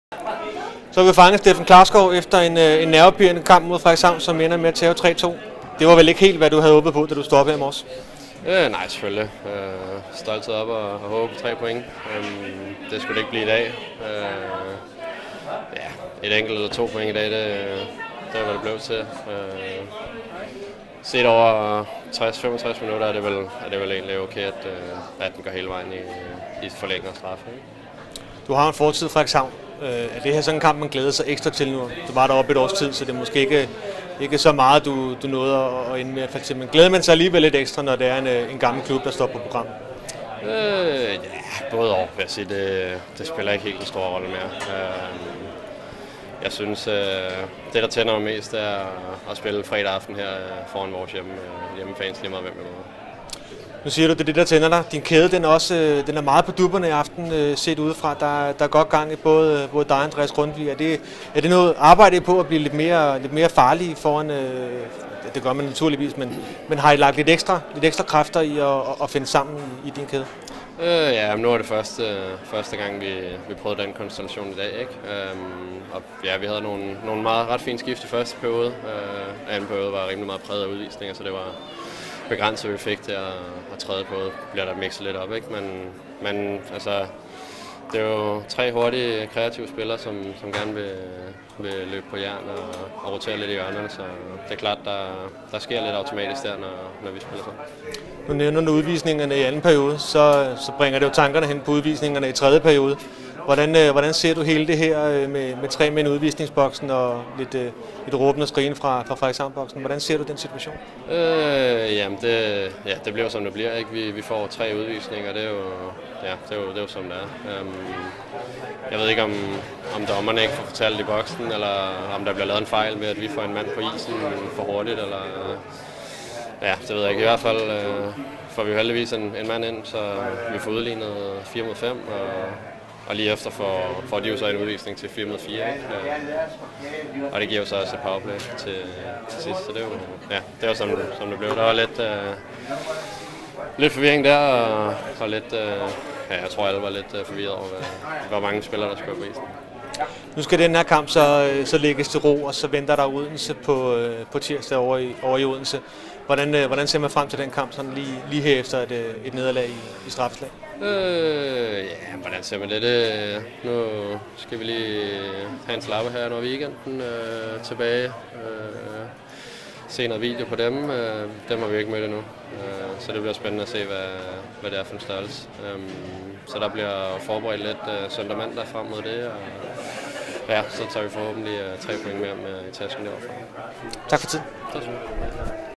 Hockeymagasinet TV Steffen Klarskov
Steffen Klarskov efter 2-3 imod Frederikshavn